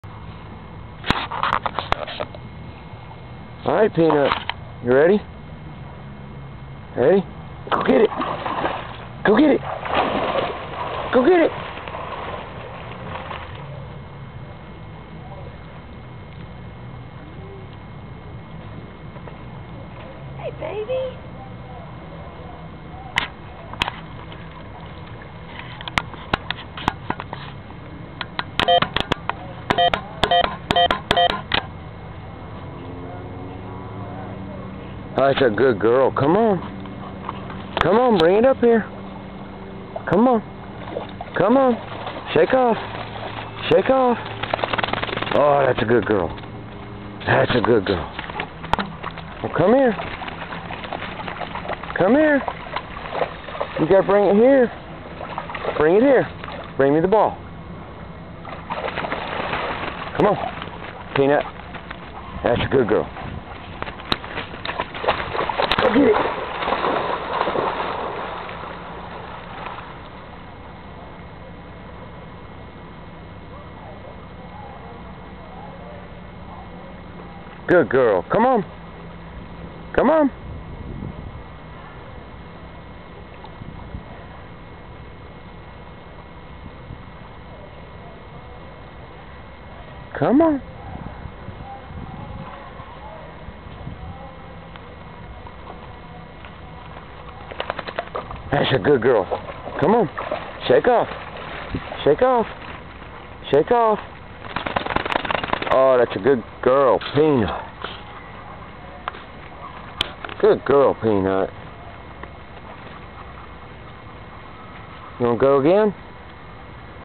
Peanut's 1st Swim
Peanut on her first swimming trip at Lake Allatoona